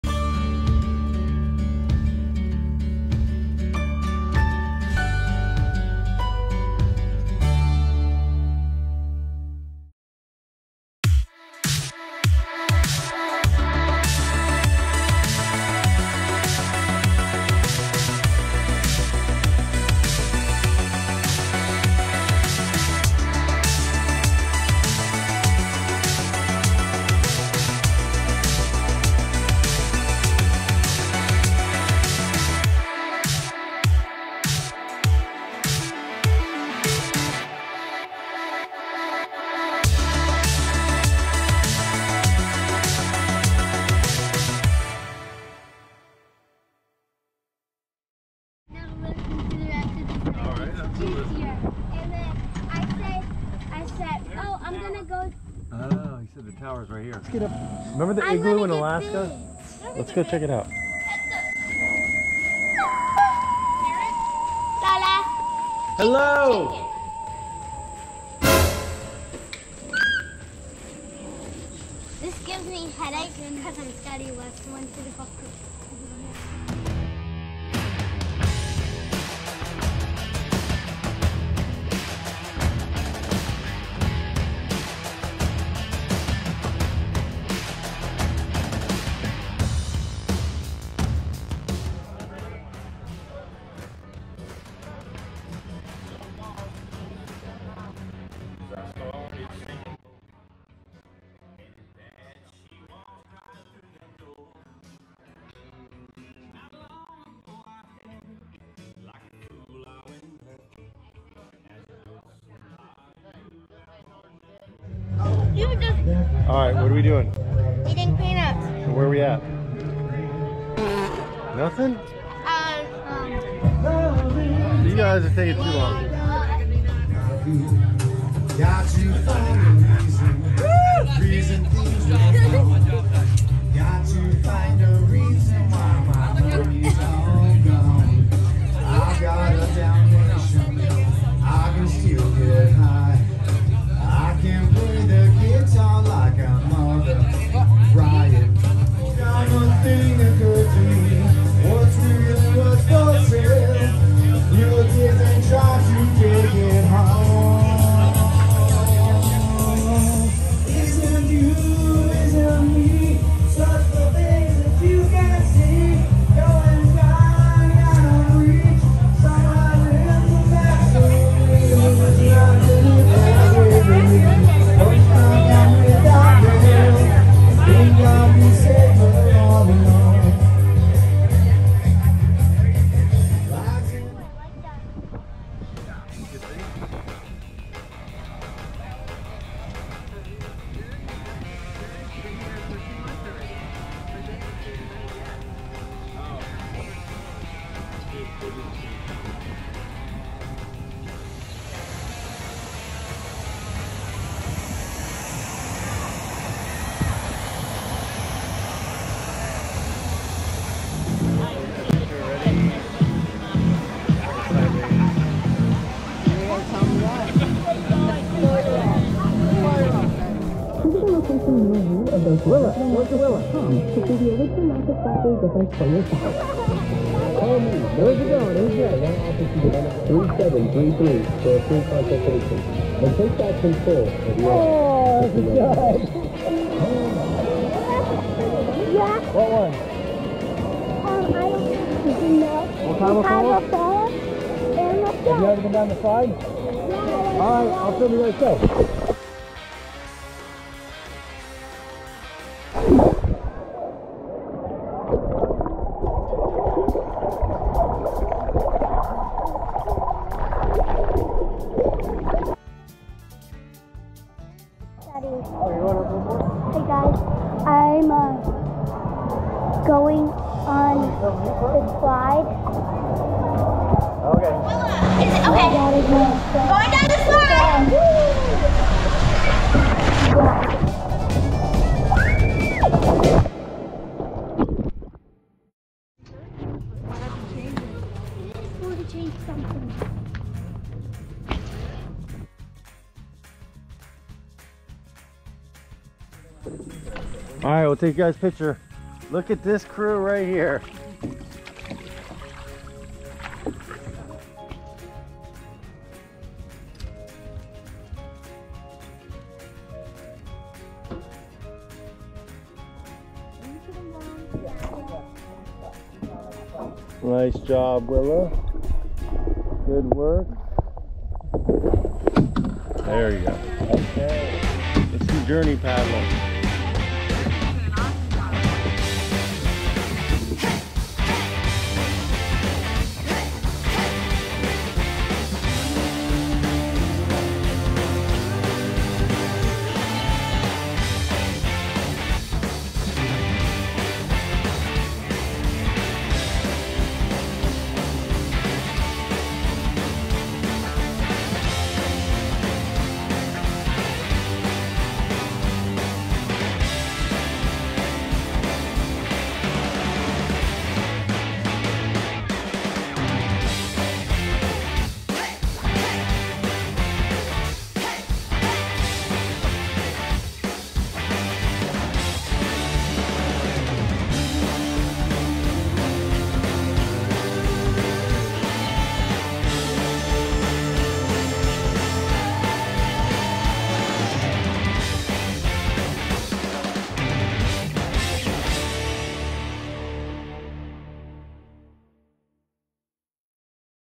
Wekiva Falls Rv Resort weekend Florida Summer camping trip
The Nolli family and Journee Cooper spent a weekend in July Summer 2021 camping in out Thor Vegas 24.1 with friends.